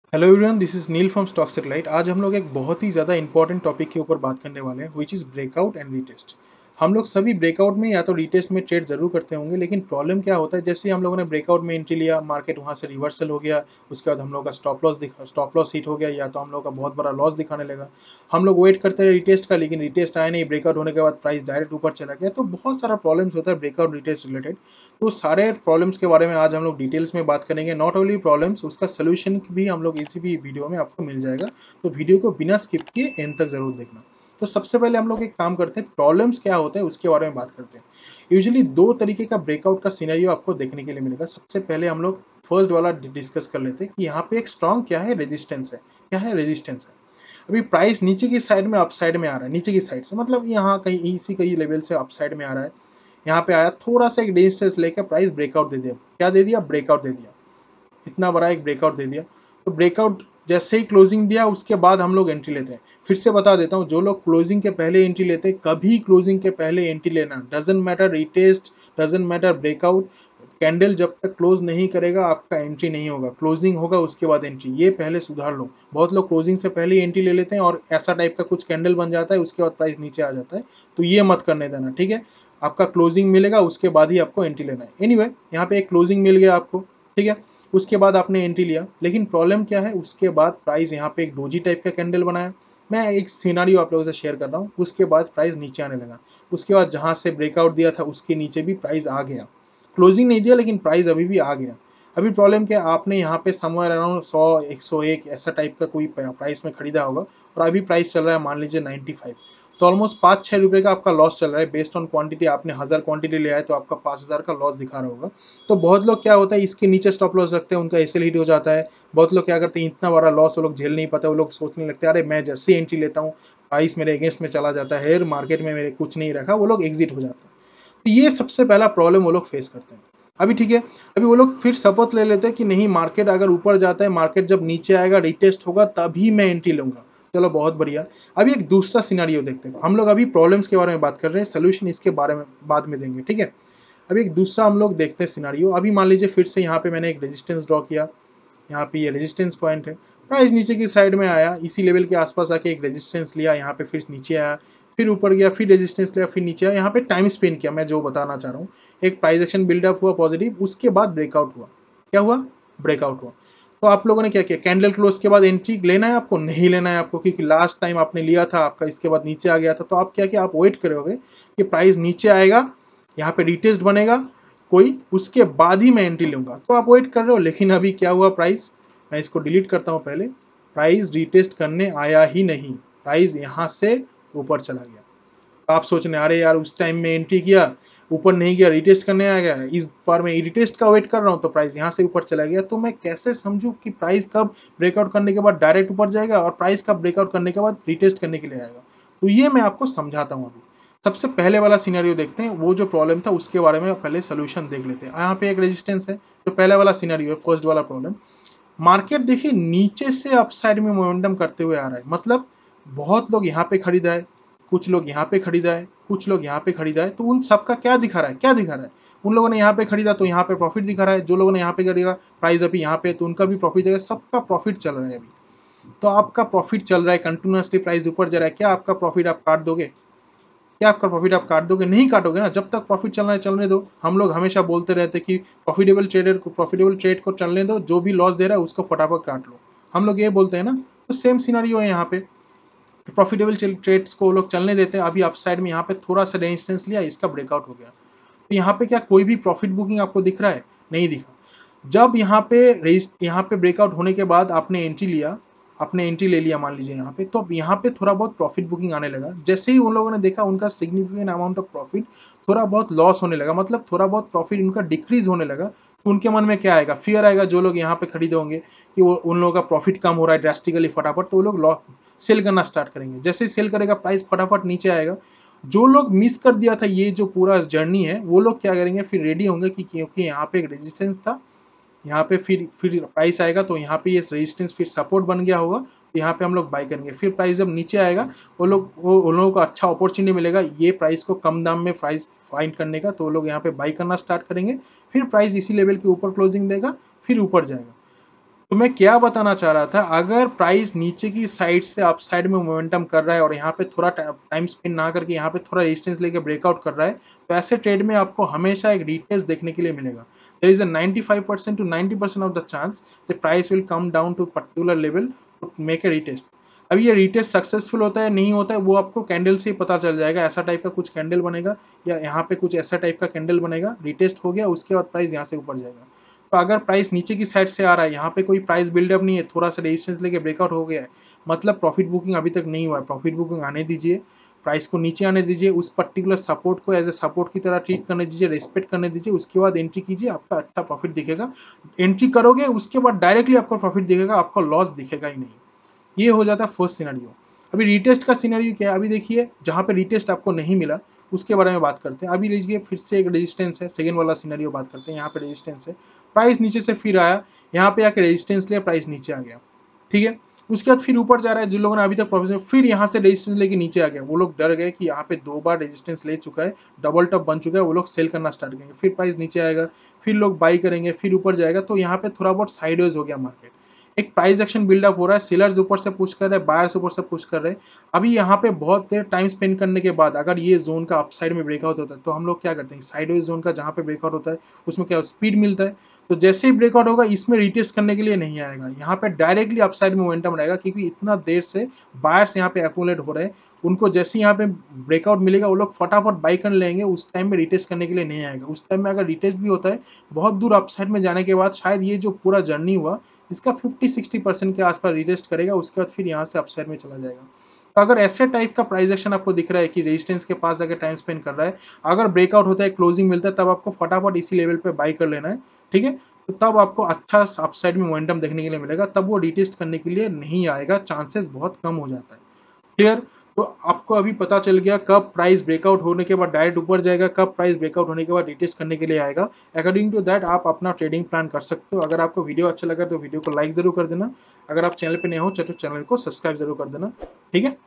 The Ultimate Guide To BREAKOUT & RETEST
In this video we’ll walk you through:
How does the Breakout & Retest Happen?
Know About False Breakout!
How to Trade Breakout?
How to trade Retest?
When will the Price Come for the Retest?